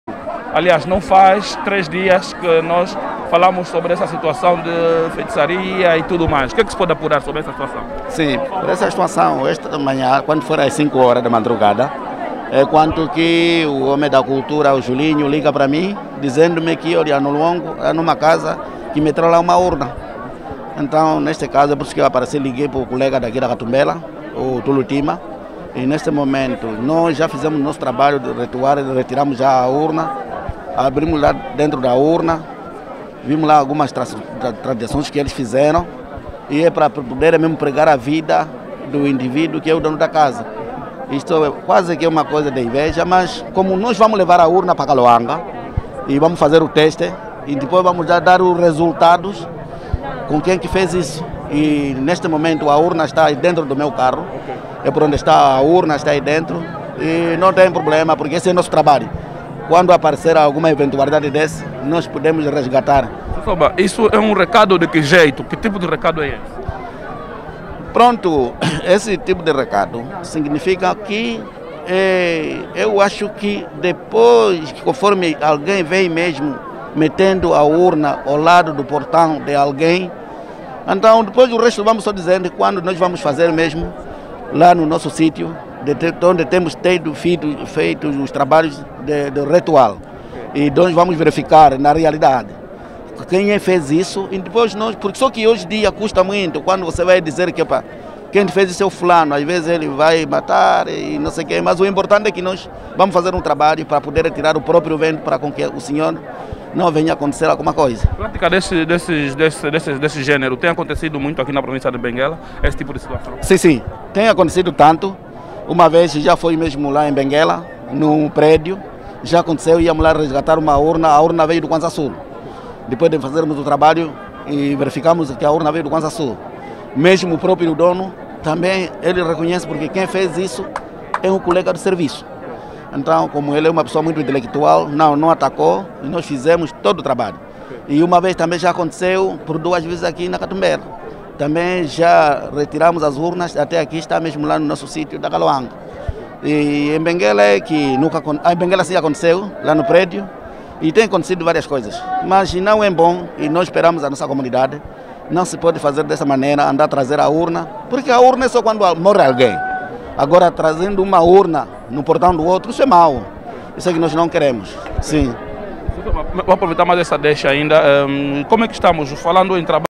Caixão é colocado no portão de cidadão, Em Benguela
Pelo que consta, na casa de um tenente coronel, em Benguela, foi colocada um caixão como símbolo de resgate à dívida, ou ajuste de contas, dos kimbandeiros com a esposa do senhor.
ela, segundo os moradores, fazia serviços obscuros para segurar o marido e não concluiu o pagamento, enfim, os soberanos tradicionais vão resolver e apurar os factos.